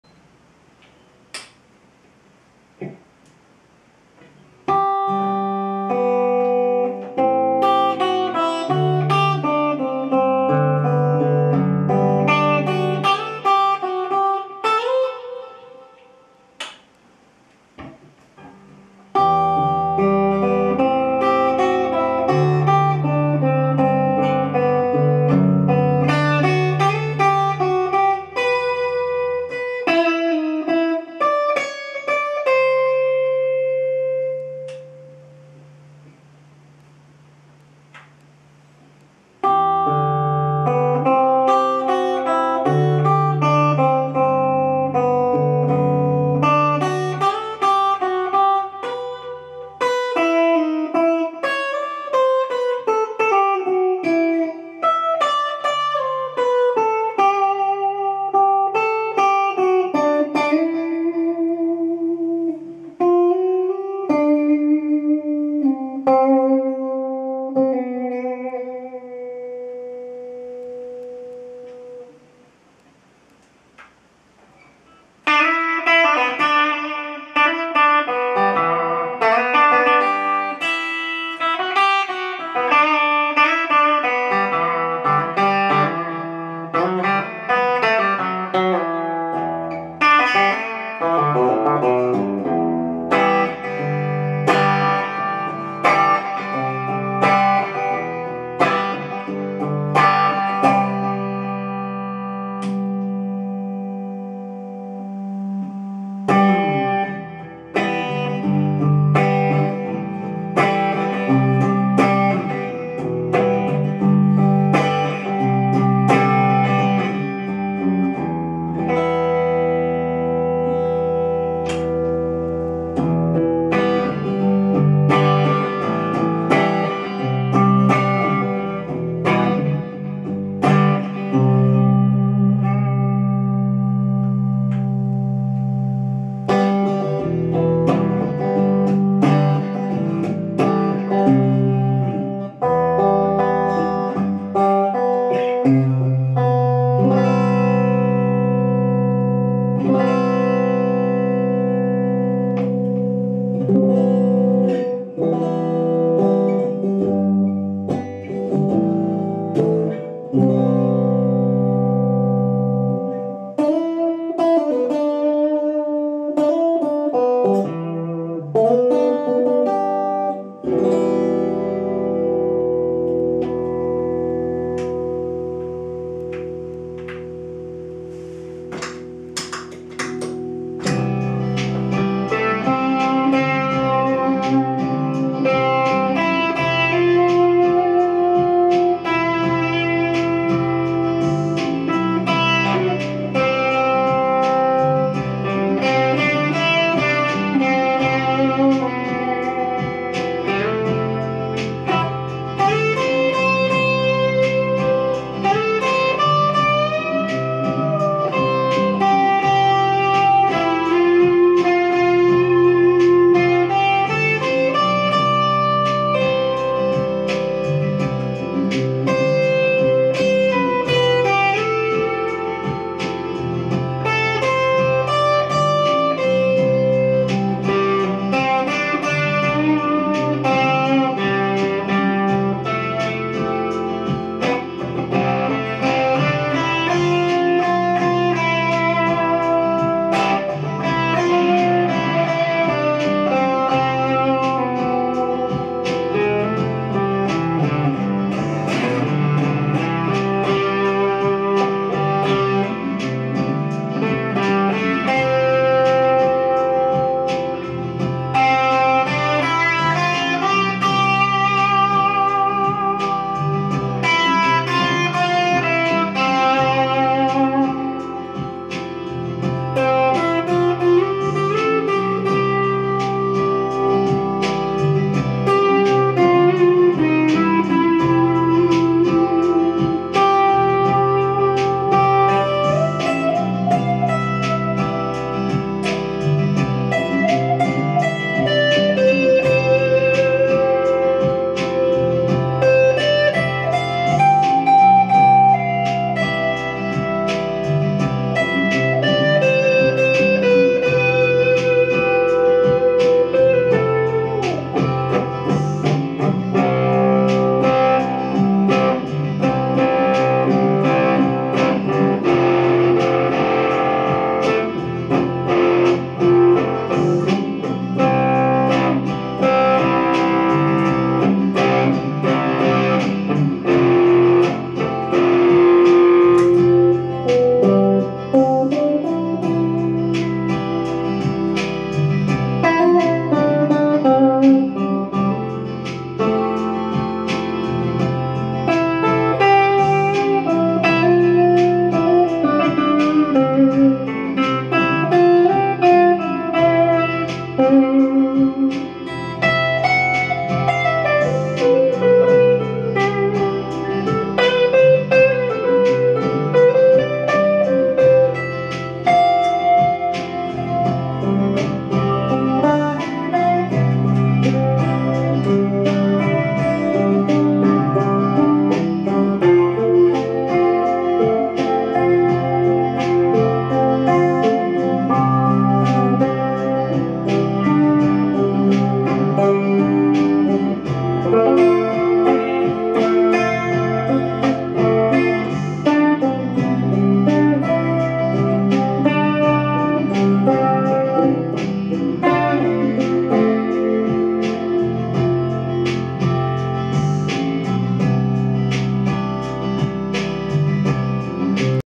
Telecaster Bakelite La Cabronita Especial All Maple Birdseye 3PUP
Muestra de sonido de Telecaster Bakelite La Cabronita Especial All Maple Birdseye 3PUP.